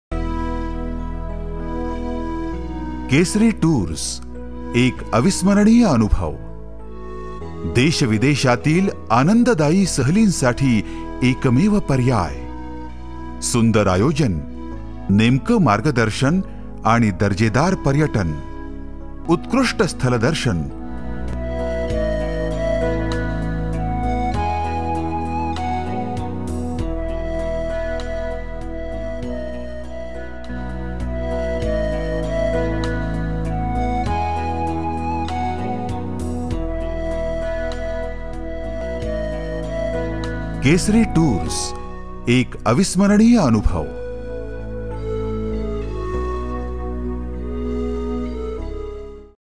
Canada - A kaleidoscope of vibrance with KESARI
Create a travelogue sprinkled with the vibrance and colors of Canada! Listen closely to the whispering of the Niagara falls and experience the unraveling of nature's various moods. Sunny yellow once and a cool shade next, Canada is sure to enchant you into coming back again and again. And who better a travel companion than Kesari!